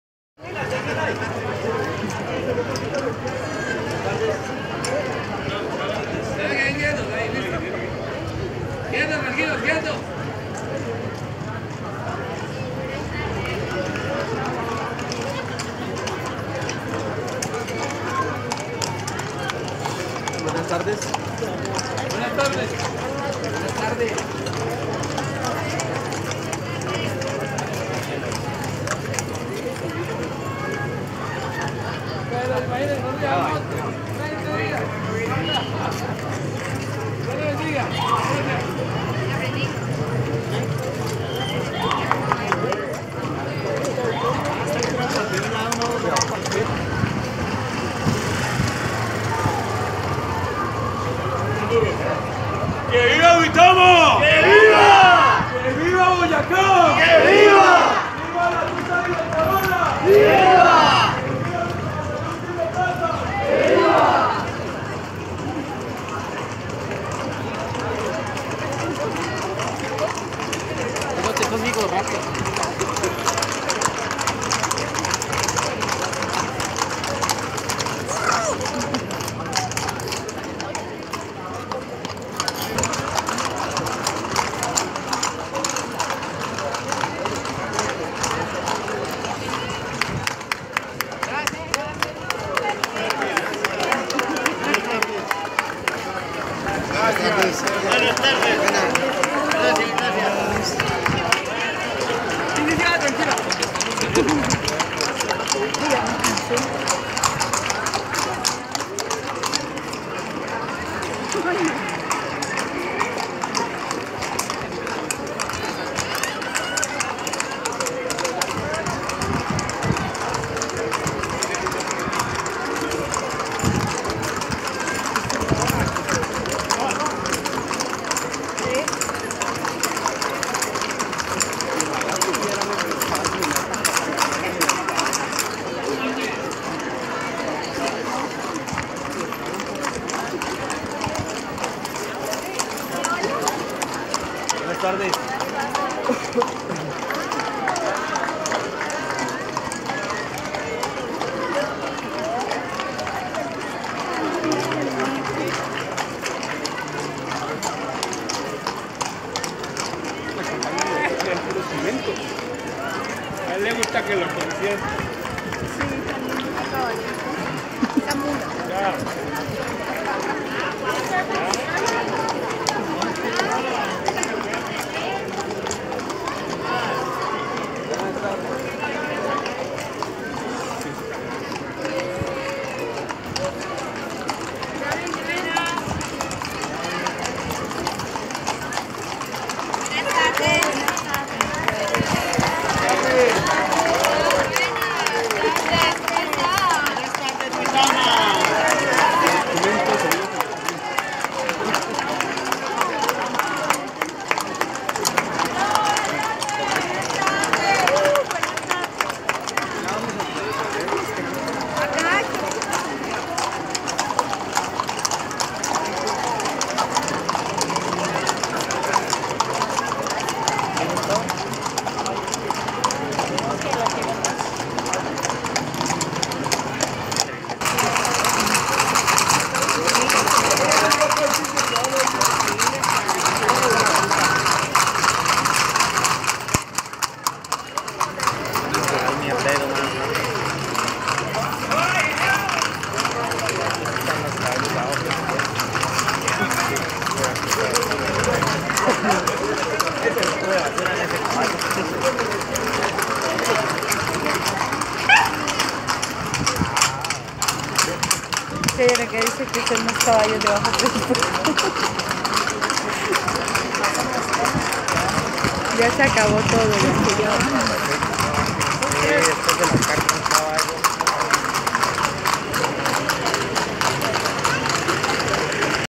Ruta Libertadora Duitama
Desfile Ruta Libertadora Duitama - Boyacá - Colombia
Julio 2019